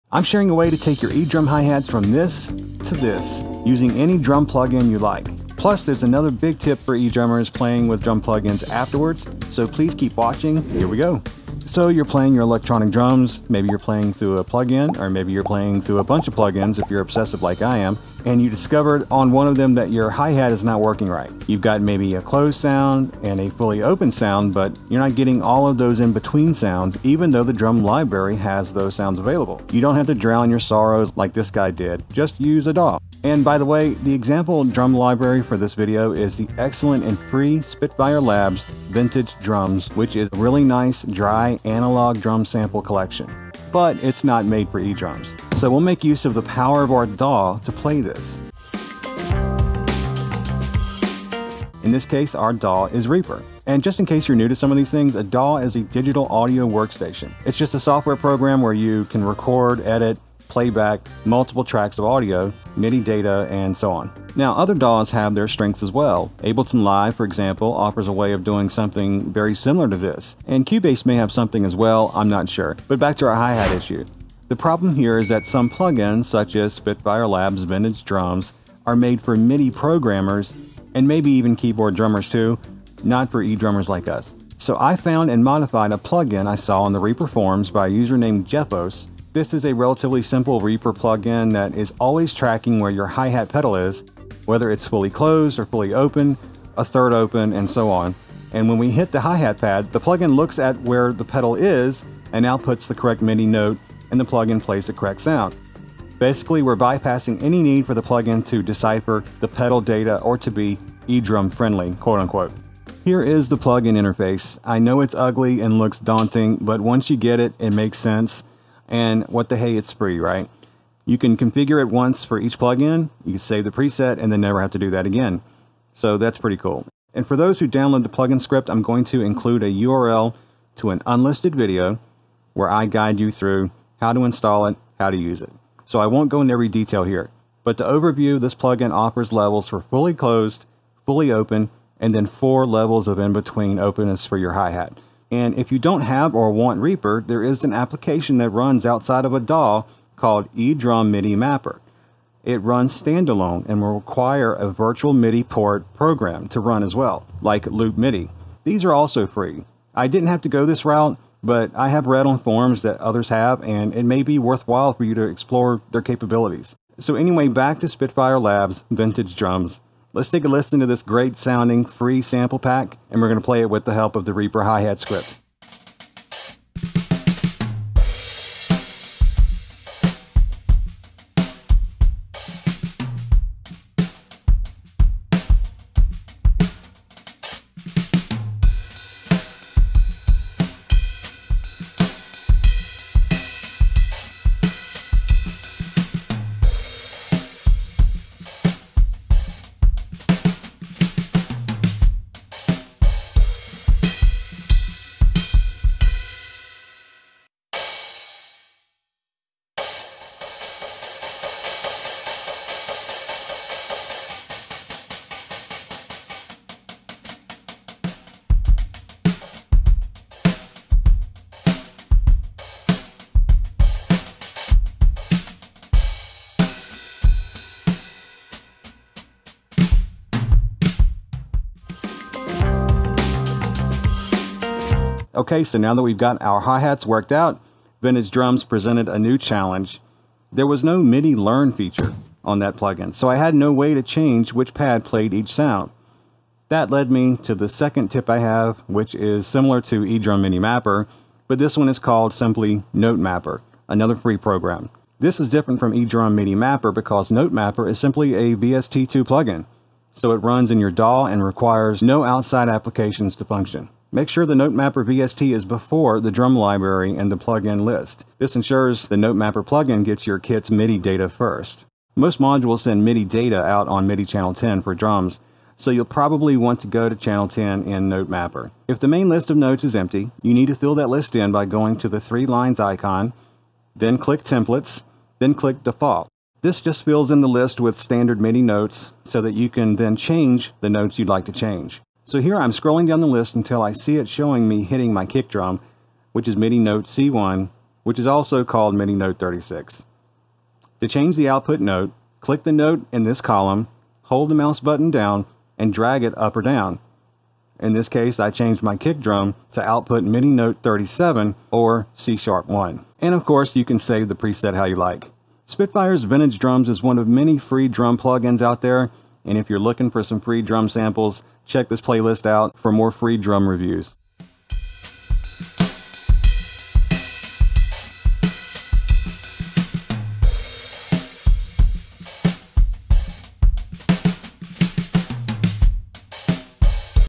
Make Your eDrum HiHat WORK with ANY DRUM PLUGIN/SAMPLER
It's just a ZIP file with two files inside: the reaper plugin file, and a short "README.txt" file which tells you how to install it and how to watch the guide video I made to get you started.

*** VIDEO DESCRIPTION***
My electronic drum hihat works with any drum sample library or plugin I want, and I'll show you how to do the same with your kit. I also cover other key apps that will make connecting your edrums to any drum VST a much more seamless operation than it may have been before. In this demonstration, I show the excellent and FREE Spitfire Labs "Vintage Drums" plugin, which runs as a VST/VST3 in your DAW.

Thanks for coming to the channel! Please check out the other videos for more drum sample library reviews and other electronic drum tips.

eDrum MIDI Mapper (not my app, its a free program though)

NoteMapper (a free VST plugin for your DAW, and also not my program)

Drums: Roland TD17kv
Samples: Spitfire Labs FREE Vintage Drums
Drummer: Faceless Drum Guy
Narrator: Faceless Drum Guy's aloof twin brother
Editor: Faceless Drum Guy's twin cousin

*** CHAPTERS ***
0:00 - What this is
0:11 - The problem
0:33 - The FREE drum sounds we're reviewing
0:50 - Reaper, DAWs
1:15 - The Reaper plugin/script to fix our edrum hihats
2:17 - What the script does
2:25 - Non-Reaper options
2:51 - Let's play Spitfire Labs Vintage Drums!
3:52 - The second big edrum tip